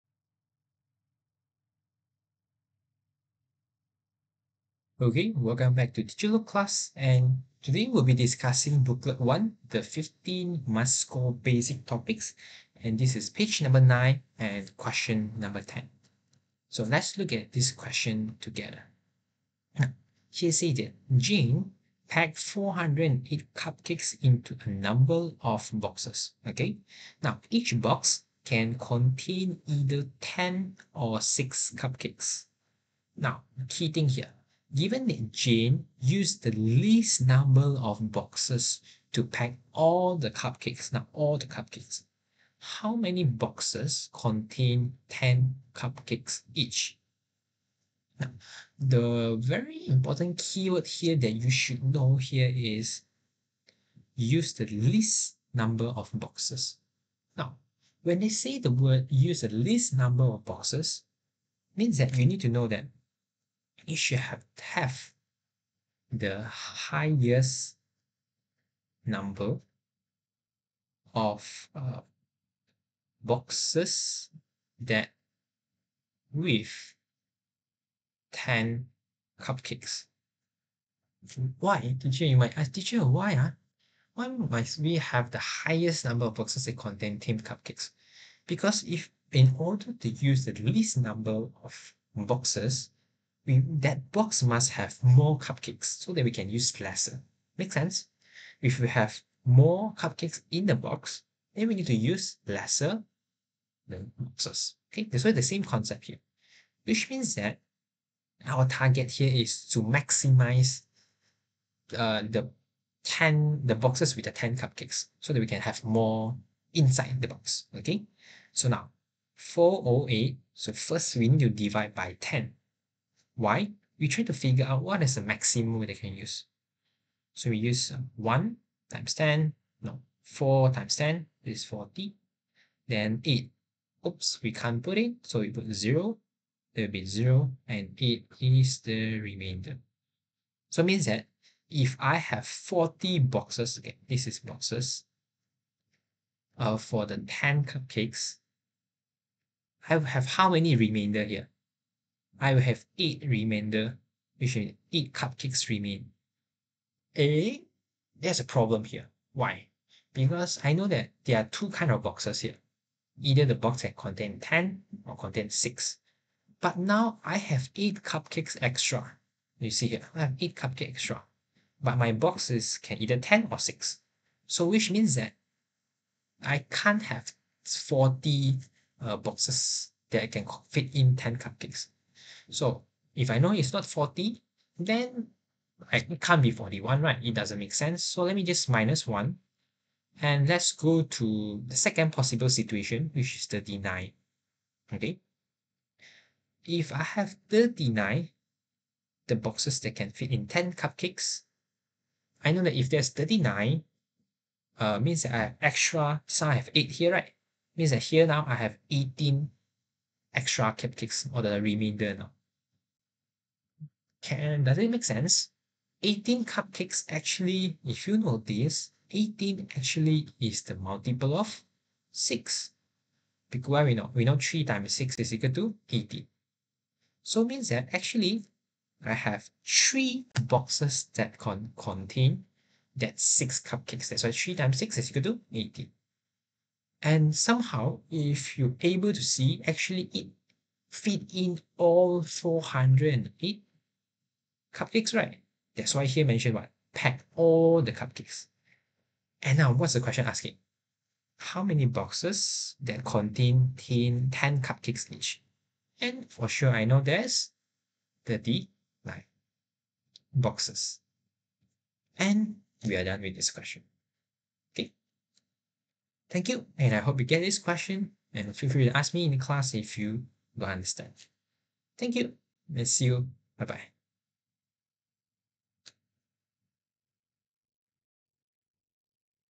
Booklet 01 Page 9 Question 10
Please log in to our Google Classroom to get the booklet note, "01 The 15 Must-Scored Basic Topics".

The purpose of the video is to help you to do revision in case you have forgotten the concept before the prelim or PSLE exam.